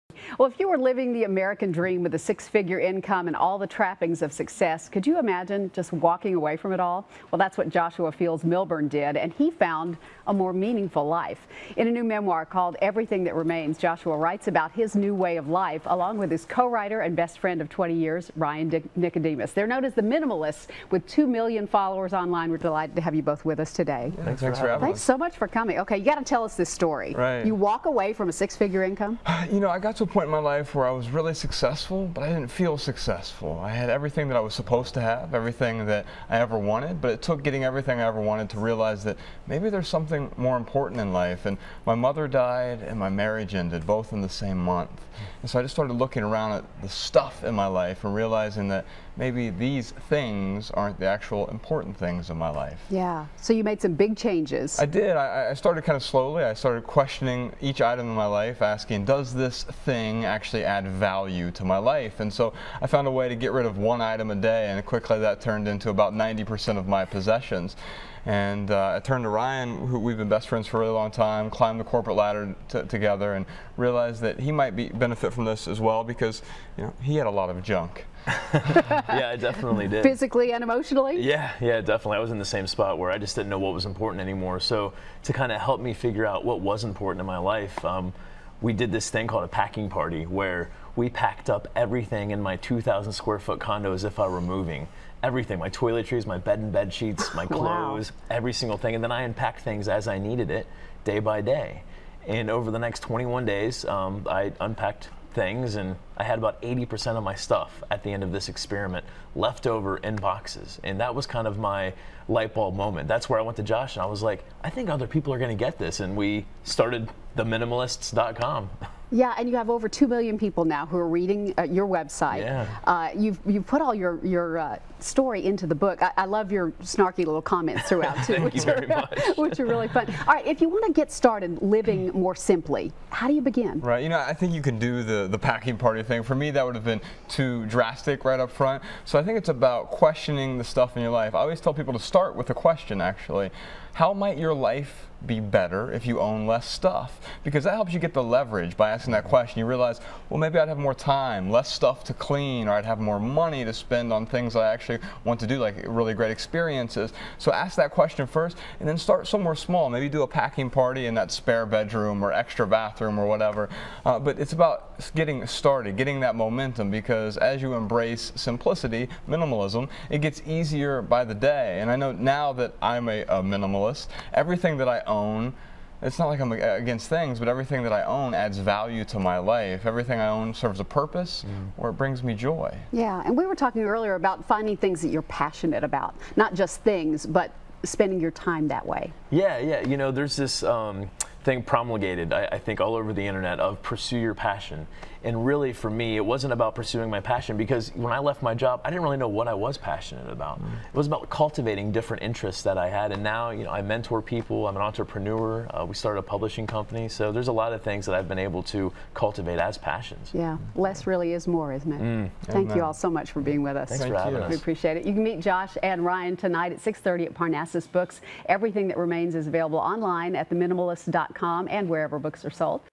Talk of the Town Authors & Books: 'The Minimalists' Talk About New Book
Authors Joshua Milburn and Ryan Nicodemus shared their new book "Everything That Remains". They are better known to their 2M+ followers as The Minimalists